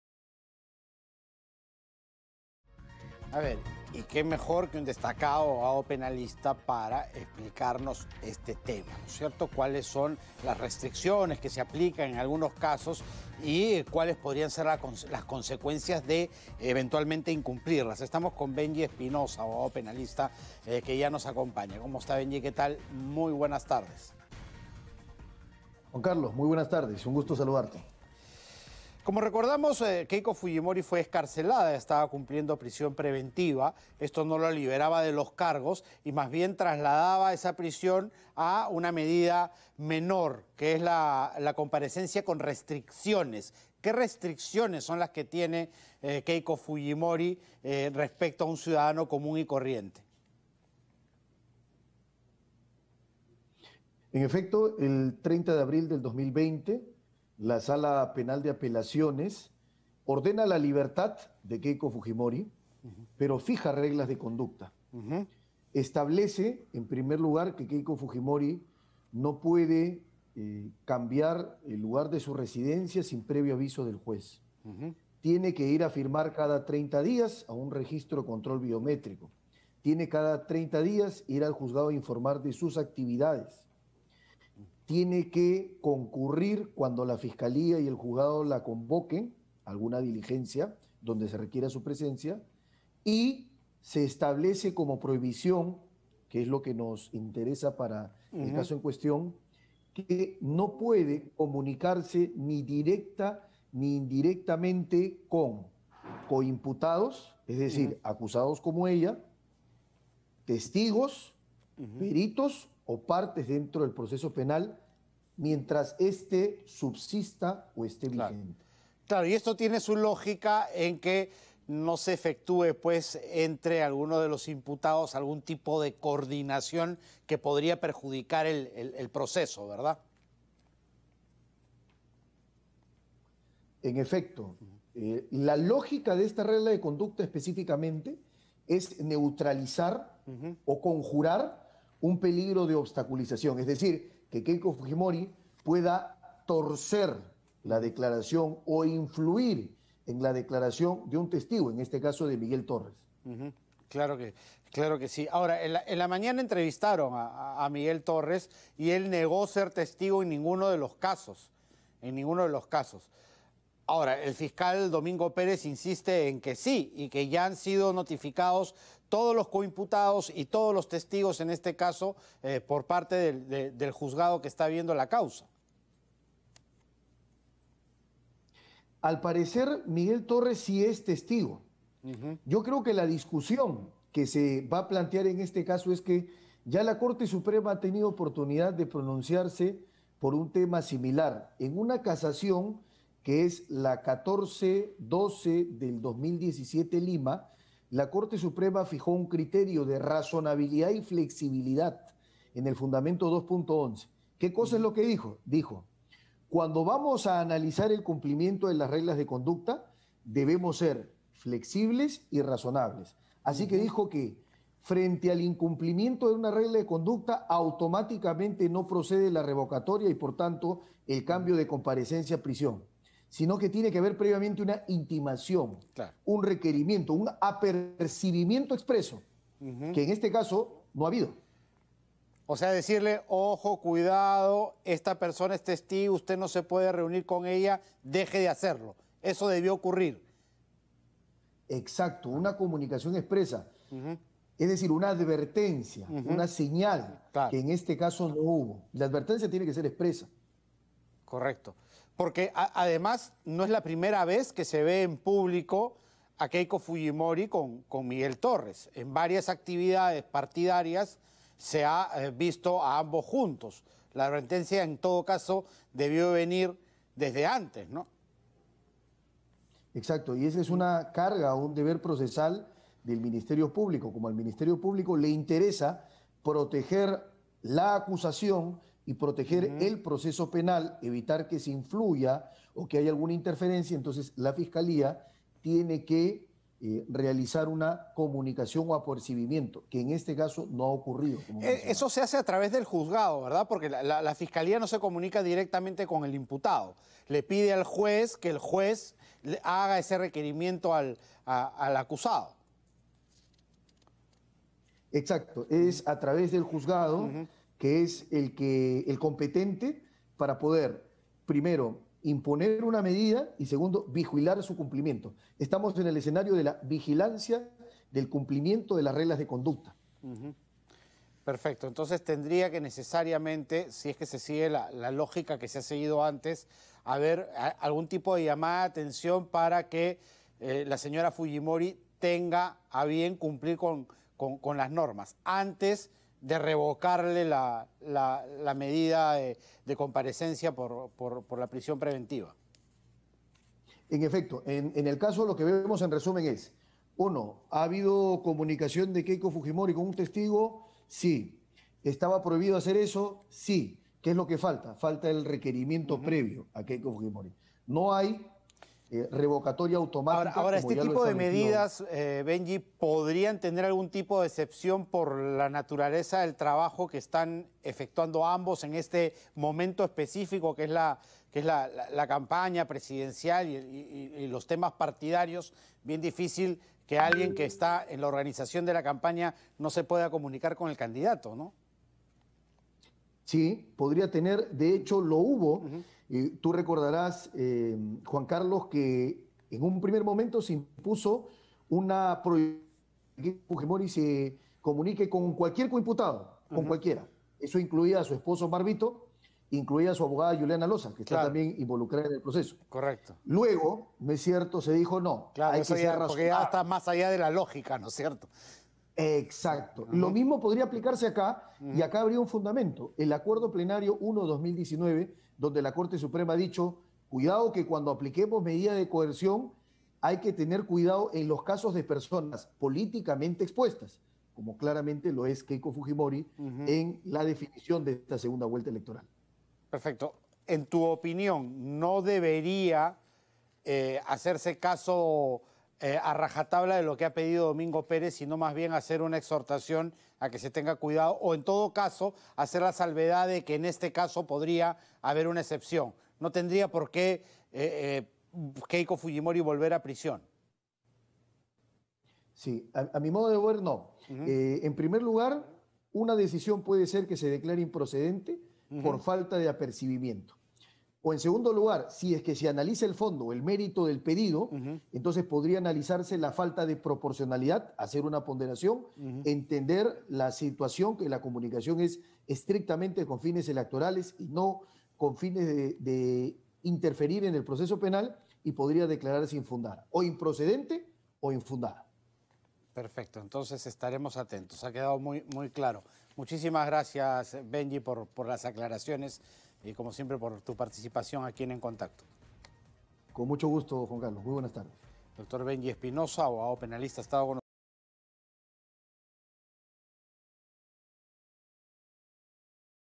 VARIACIÓN DE COMPARECENCIA POR PRISIÓN DE KEIKO FUJIMORI - ENTREVISTA EN ATV+
Entrevista realizada al doctor Benji Espinoza Ramos respecto al requerimiento de variación de comparecencia con restricciones por prisión preventiva de Keiko Fujimori.